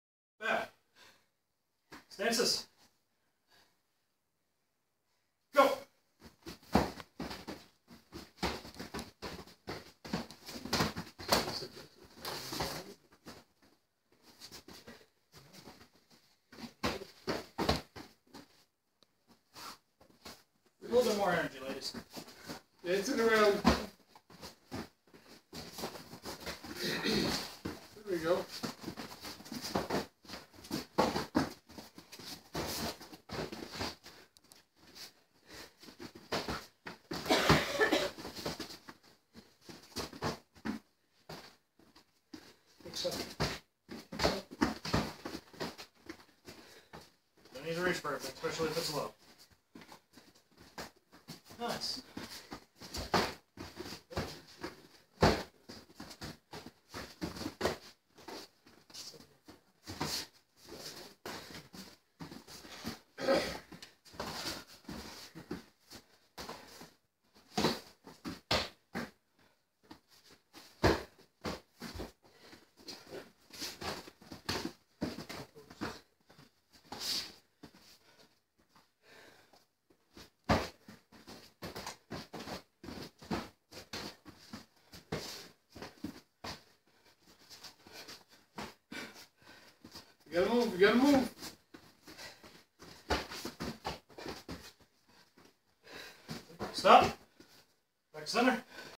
Sparring class two
Sparring. Again. Yellow belt with white helmet is me